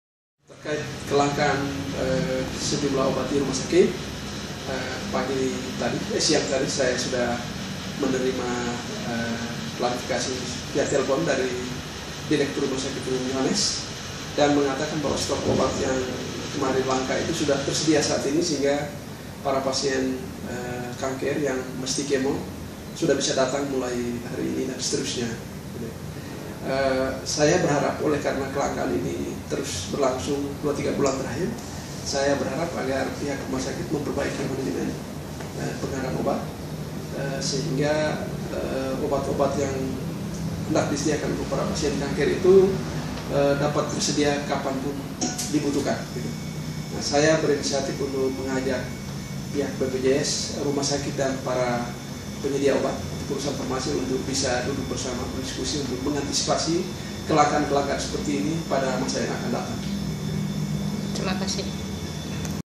Ombudsman NTT Ungkap Stok Obat di RSUD Prof Dr WZ Yohannes Kupang - Be Sante
Ombudsman NTT Soal Stok Obat di RSUD Prof Dr WZ Yohannes Kupang,  Januari 2017-  Be Sante

Dalam kesempatan ini Kepala Ombudsman Perwakilan NTT, Darius Beda Daton, SH, menginisiasi pertemuan BPJS, Rumah Sakit dan perusahaan Farmasi, Senin (23/1/2017).